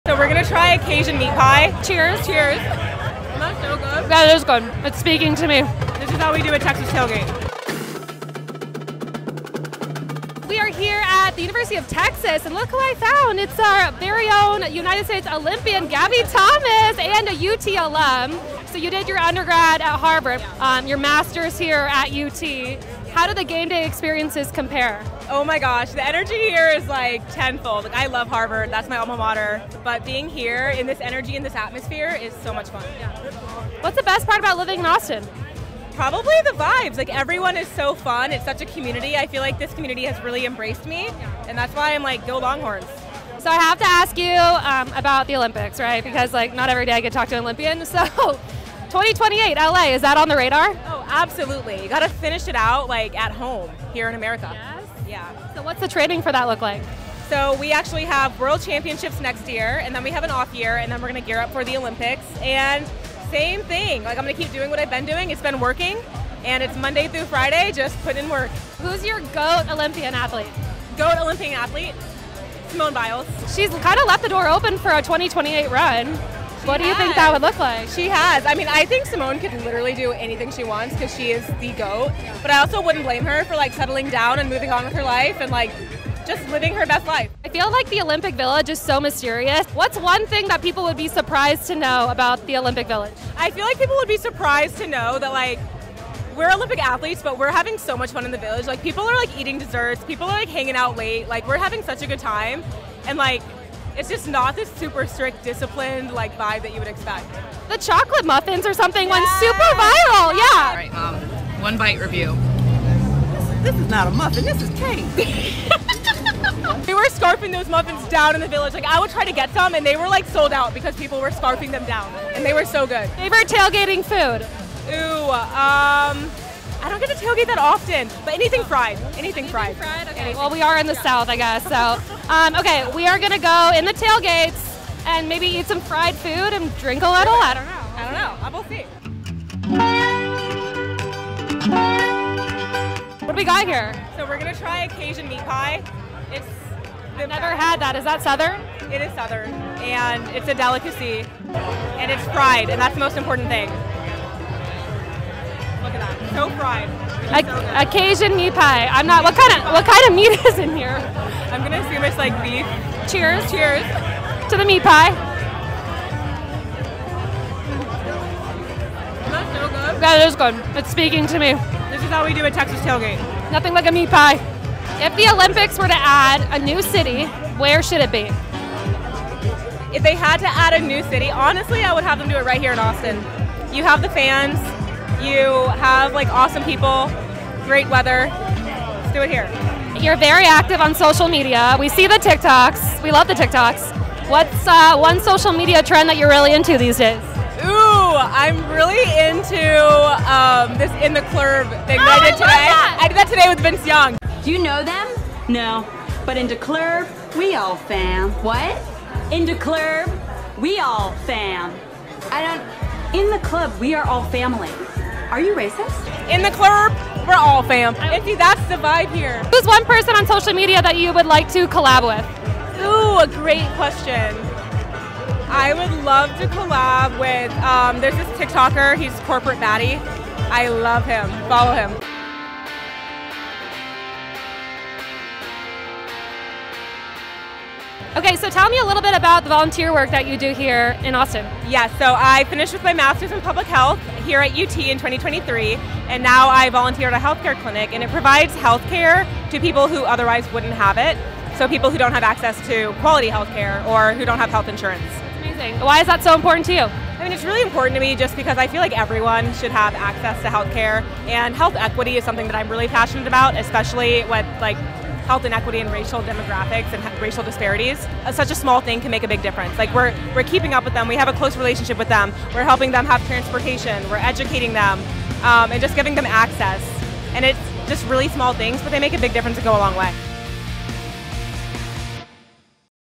Gold Medalist Gabby Thomas Shines at Texas College Football Tailgate!
Three-time Olympic gold medalist Gabby Thomas joined Sports Illustrated at a Texas football tailgate and shared her journey toward the 2028 Olympics. She opened up about her training, celebrated the incredible Simone Biles, and revealed why Texas is the ultimate destination for passionate sports fans!